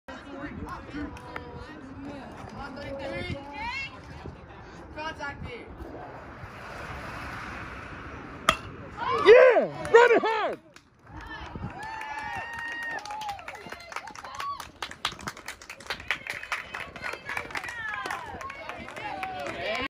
Thomas A. Edison HS Baseball Feeder Team vs. Alexandria Reds White
EJ slams one over their heads.